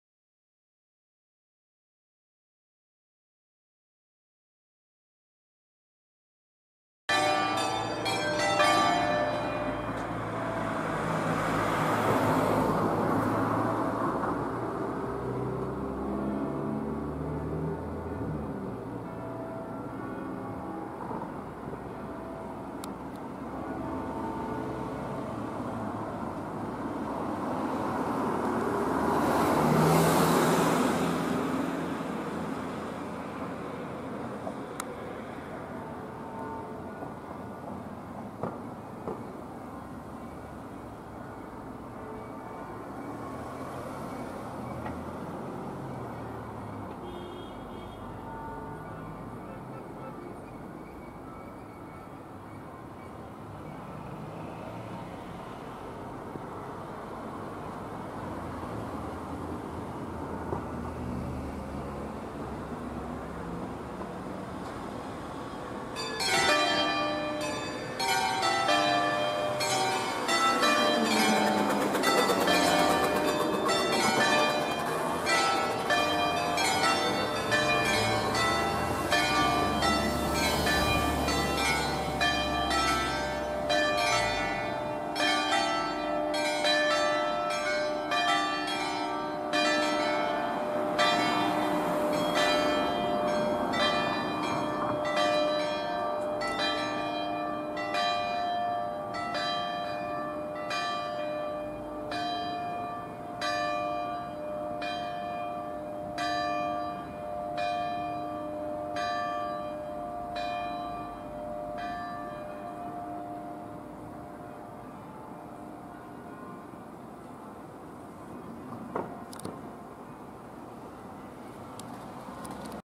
Le campane di Zogno (BG) - Fraz. Romacolo - Monastero
Zogno (Bg) Fraz. Romacolo - Convento francescano di S. Maria della Misericordia - Concerto di 5 campane in Re4 C. Ottolina di Seregno 1983 - ? per l'Angelus festivo - Note: fino a qualche mese fa queste campane non suonavano mai perchè l'impianto è troppo vecchio e le suore non avevano abbastanza soldi per cambiarlo. Quanto meno le campane ora suonano anche se la suonata è indecifrabile. Per chi non l'avesse capito ad inizio video le campane vanno tutte a bicchiere in 3 colpi. Ovviamente questo non sarà l'ultimo video postato in rete su queste campane anche perchè devo indagare sul tipo di suonata e i lavori eseguiti.